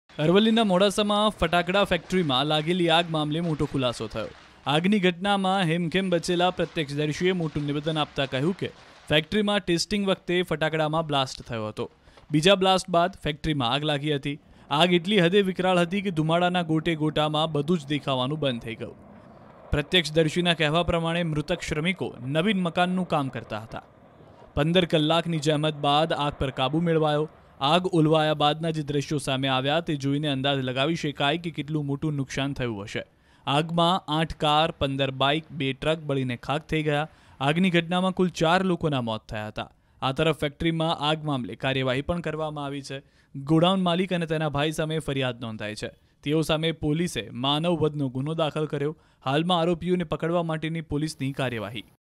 Fire Incident In Modasa: મોડાસામાં લાગેલી આગ બાદ તપાસનો ધમધમાટ તેજ | News In Gujarati | News18
After the fire in Modasa, the investigation intensified, a complaint was registered against the godown owner and his brother.

મોડાસામાં લાગેલી આગ બાદ તપાસનો ધમધમાટ તેજ, ગોડાઉન માલિક અને તેના ભાઈ સામે ફરિયાદ નોંધાઈ.